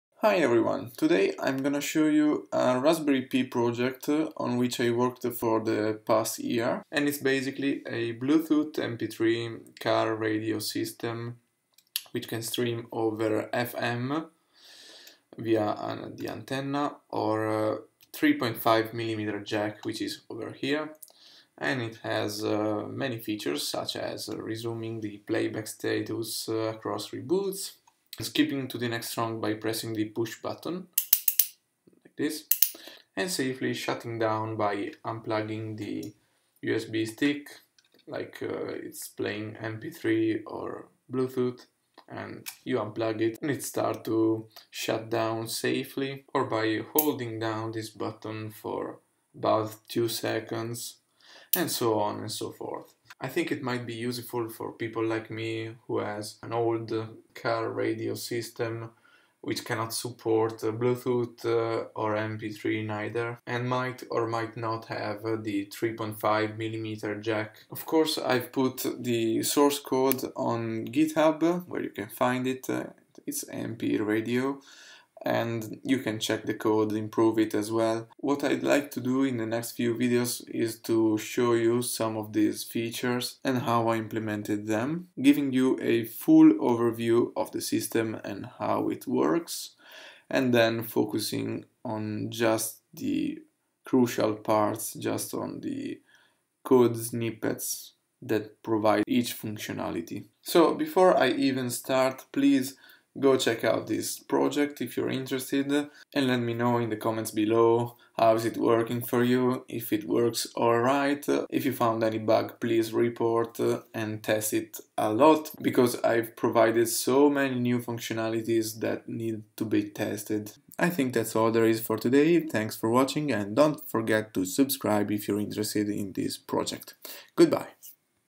Raspberry Pi adds Bluetooth/mp3 support to your car radio! [FM/3.5mm Jack]
In the past year I've been working on this project which is, indeed, a multimidia jukebox with the aim to extend functionalities to old car radio(s) by adding Bluetooth and mp3 playback, with some handy functions such as resuming the playback status of the track, skipping to the next song, etc etc.. go check it out!